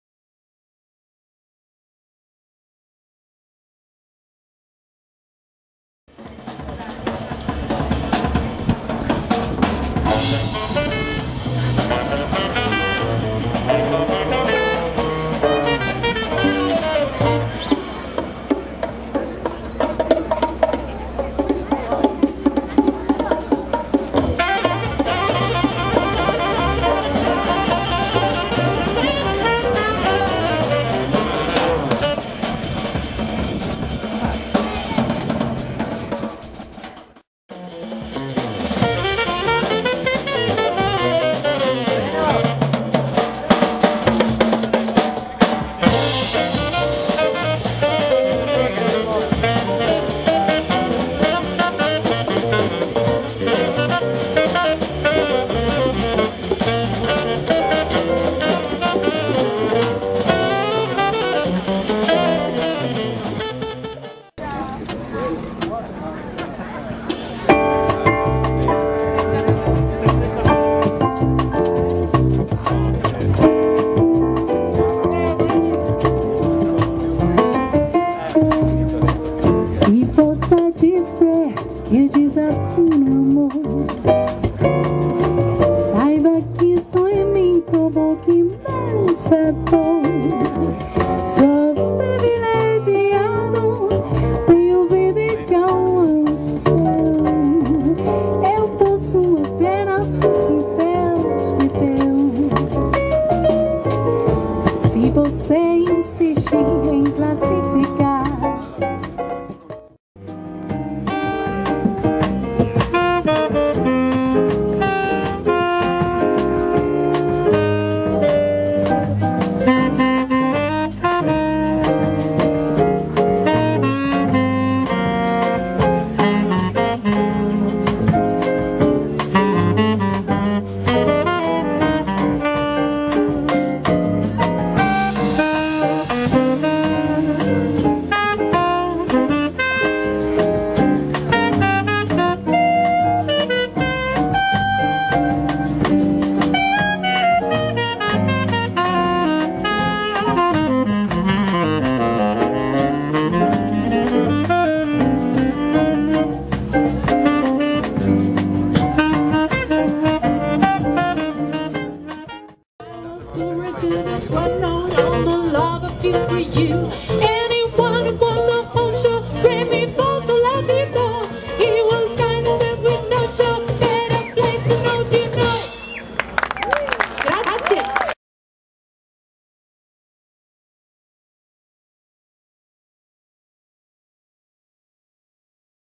VALDARNO JAZZ ENSEMBLE @ DOLOMITI SKI JAZZ 2012
Video girato durante il Dolomiti Ski jazz presso il rifugio baita Gardoné da MIPRENDOEMIPORTOVIA Travel Blog